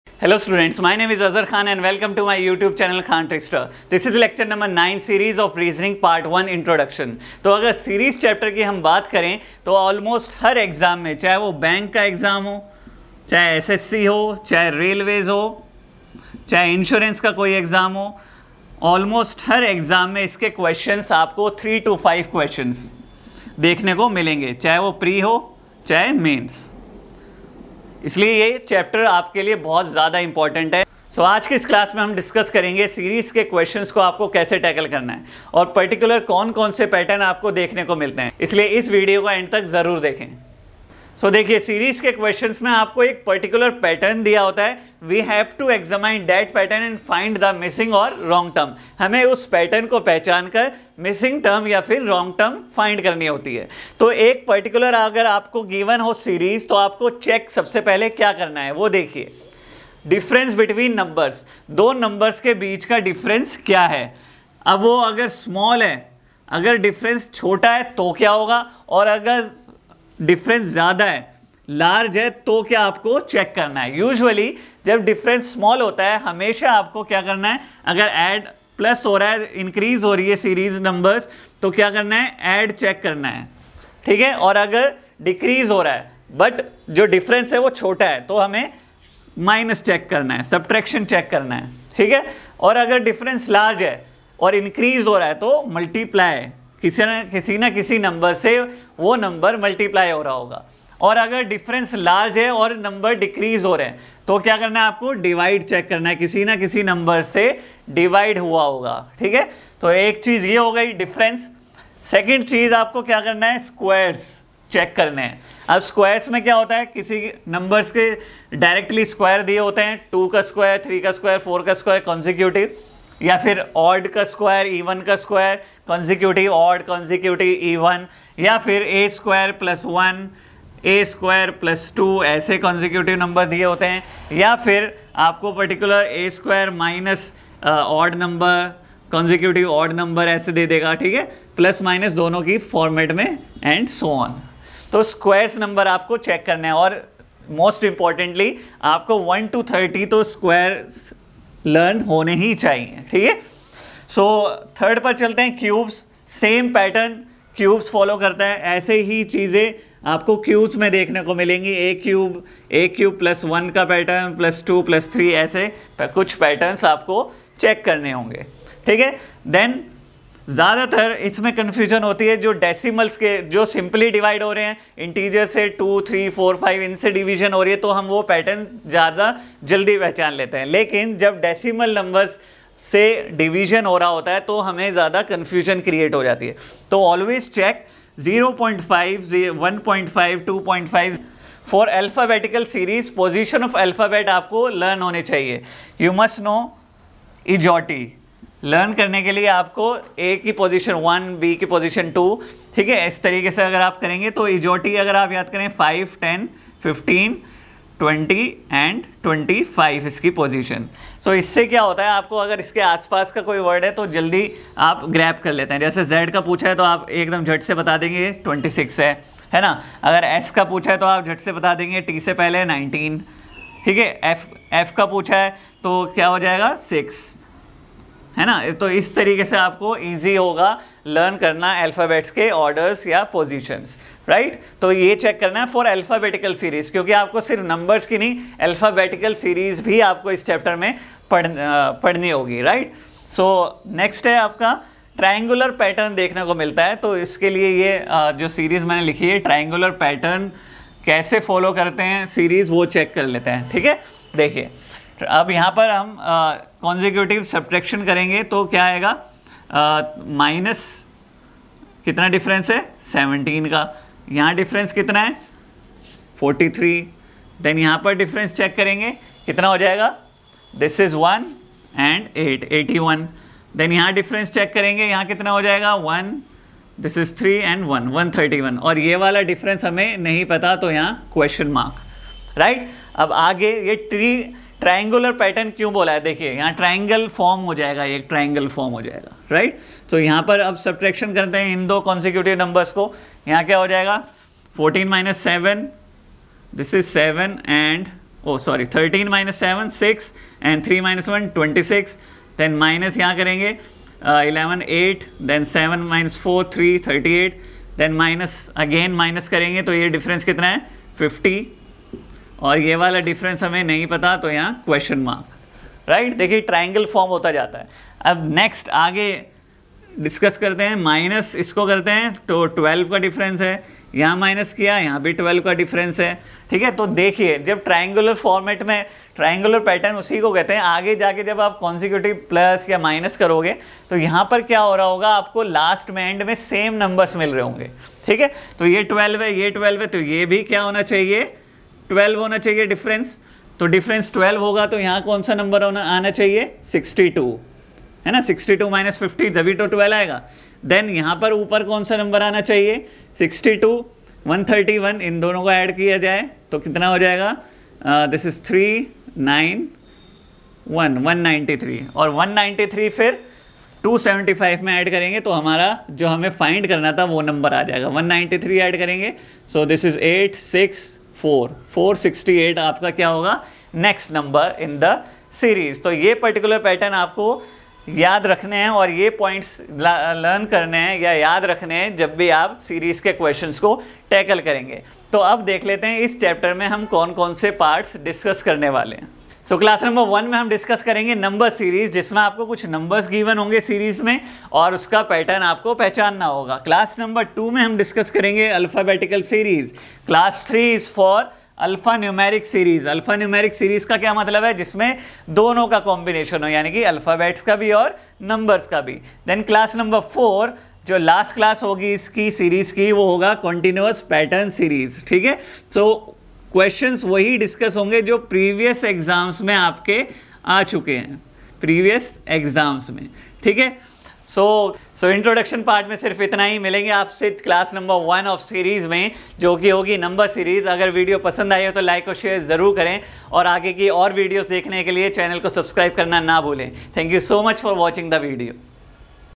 REASONING LECTURE 9 SERIES PART-1
🔘 Prepare for full Course Quantitative Aptitude and Reasoning

This video includes the introduction of the SERIES chapter in which we have discussed how we should deal with such type of question and also discussed a few important points that we should keep in mind to solve these questions.
My Gears:-
50MM LENS
CAMERA TRIPOD
LIGHT STAND